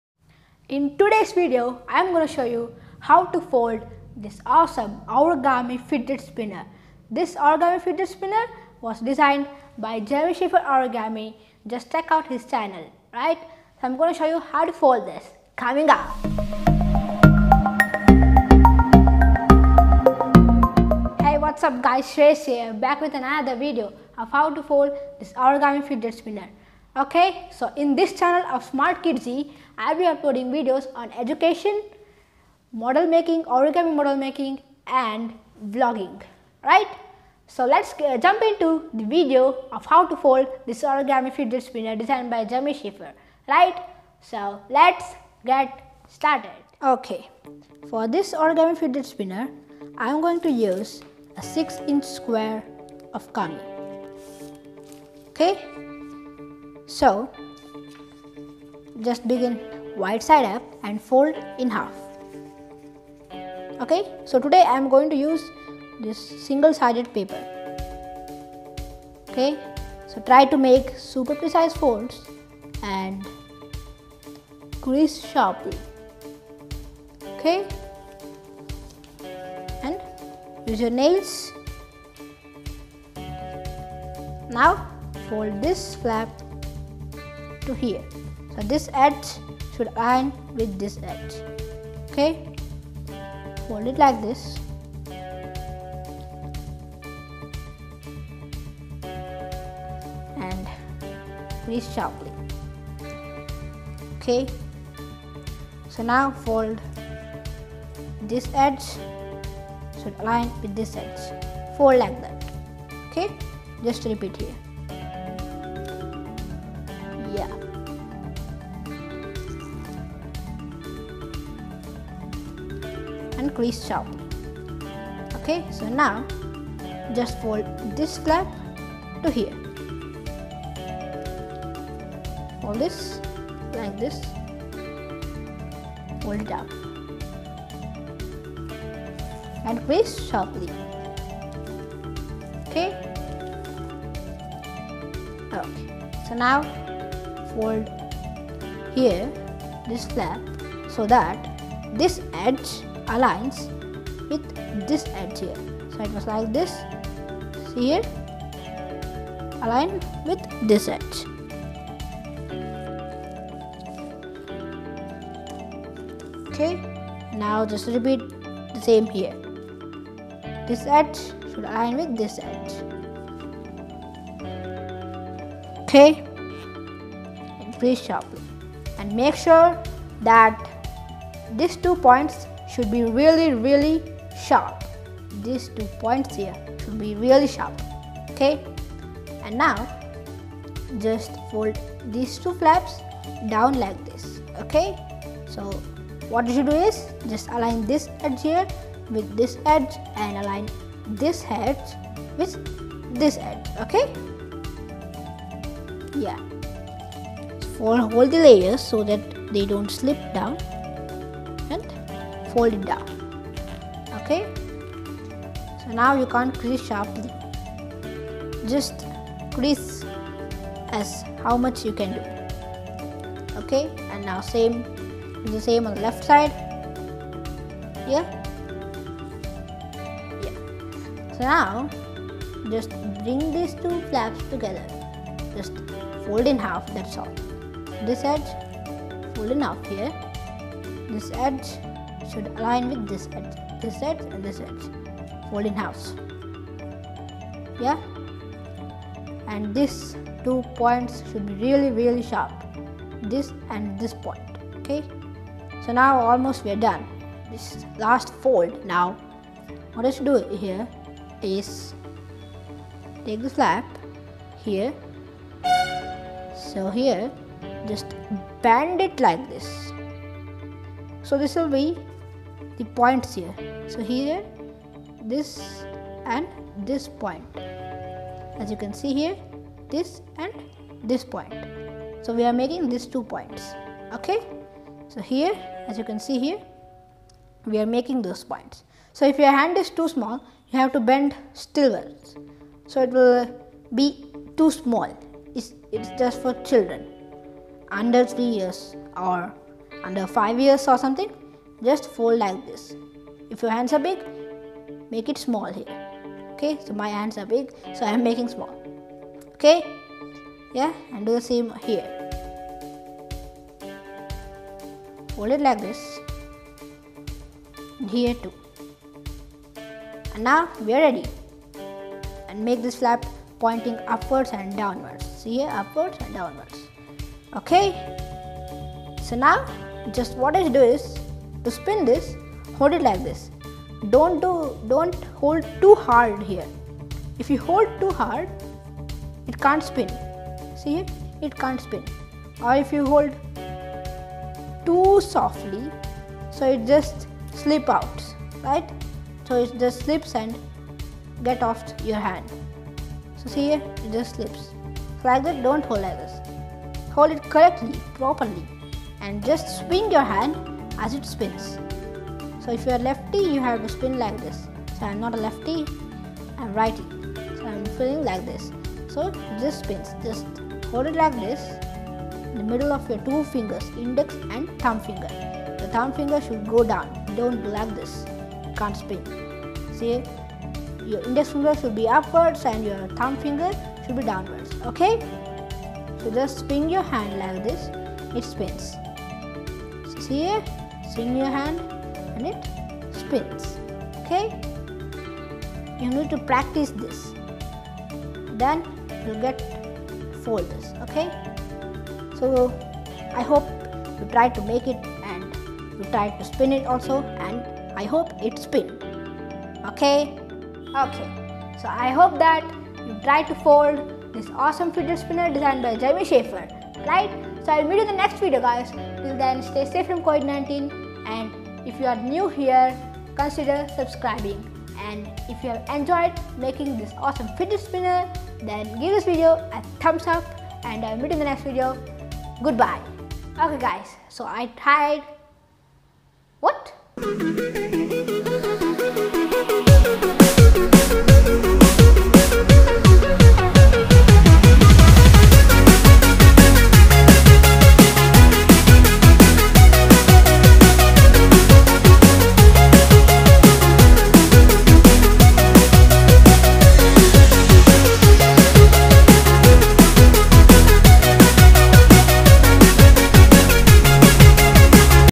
Origami fidget spinner
Hey guys, Shreyas here.
Today I'm going to show you how to fold an origami fidget spinner.
This fidget spinner was designed by Jeremy Shafer.

Regards Shreyas.